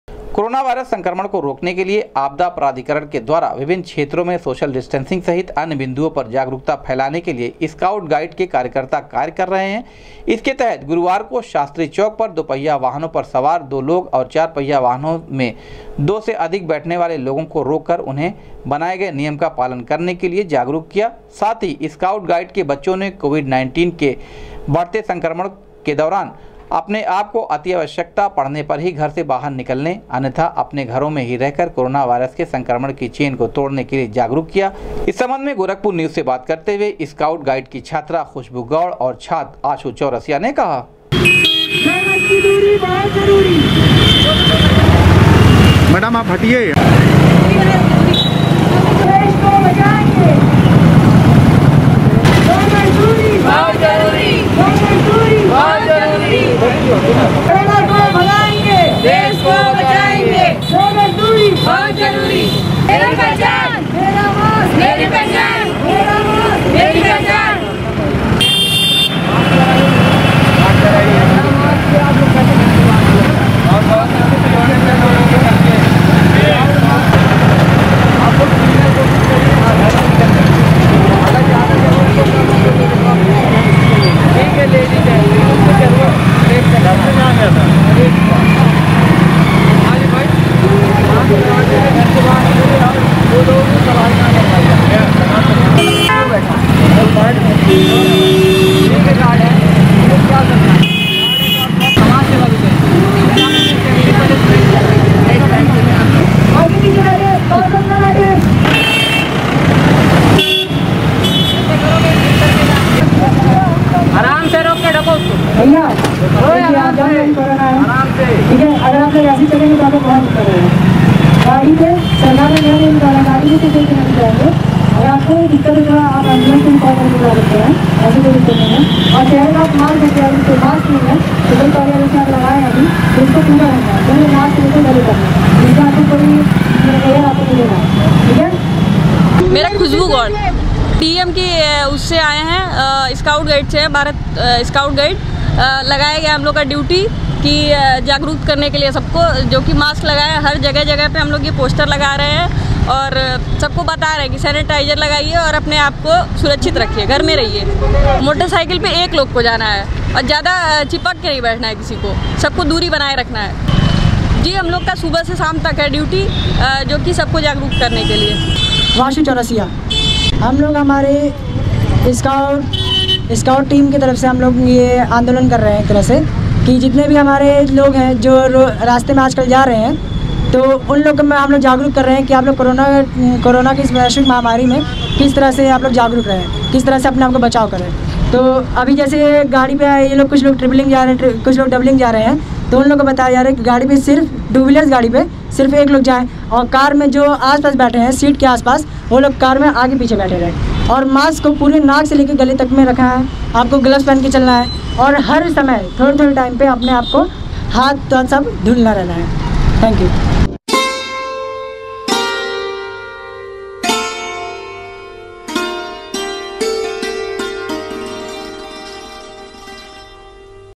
कोरोना वायरस संक्रमण को रोकने के लिए जागरूकता फैलाने के लिए स्काउट गाइड के कार्यकर्ता कर
कोरोना वायरस संक्रमण को रोकने के लिए जागरूकता फैलाने के लिए स्काउट गाइड के कार्यकर्ता कर रहे हैं कार्य, कोरोना वायरस के संक्रमण की चेन को तोड़ने के लिए कर रहे हैं जागरूक।